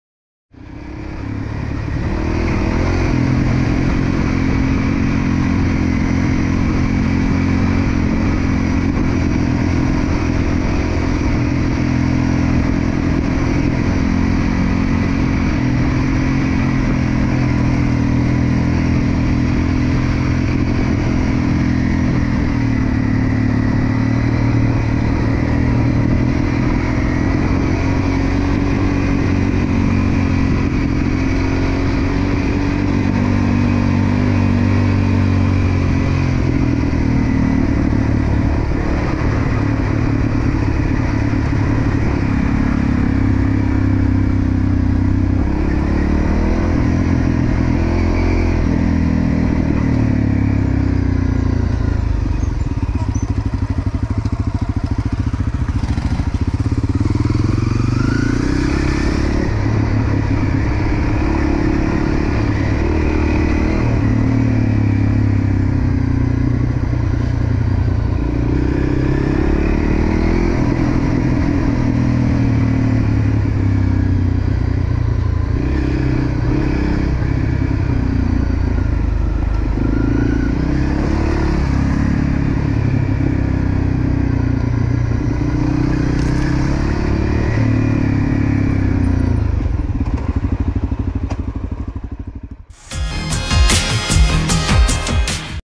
Vietnam Motorbike Tours On A Wild And Thrilling Enduro Jungle Track | OffroadVietnam.Com
Experience this wild and thrilling enduro ride in the exotic wilds of Son La, Northern Vietnam. 🌤️

Ride on a fantastic jungle trail with off-road and single tracks for the thrill-seekers. Pass through a variety of terrain and reconnect with nature. 😇

Đi đường rừng là vắng bậc nhất ace ah. 👌